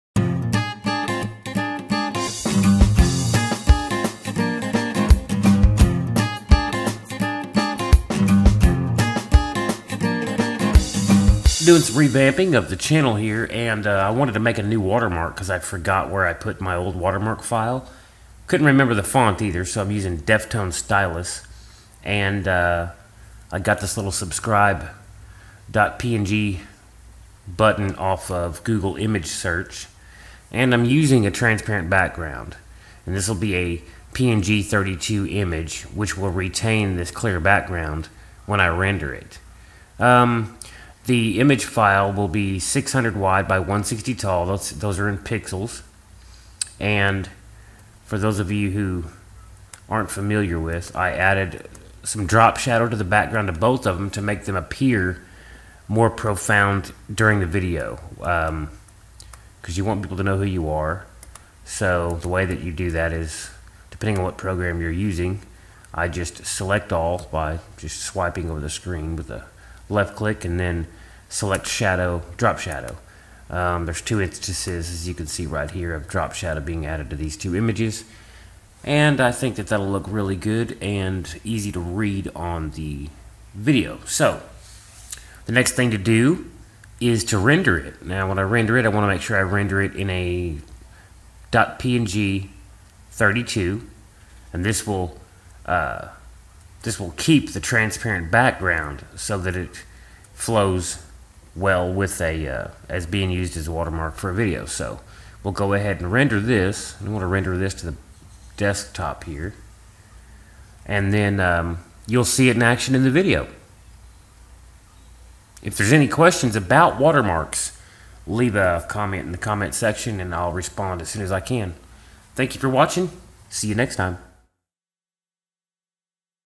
How to Make a Watermark for Videos and Images
This video will show you how to make a watermark for a video or an image. The same steps apply for both projects.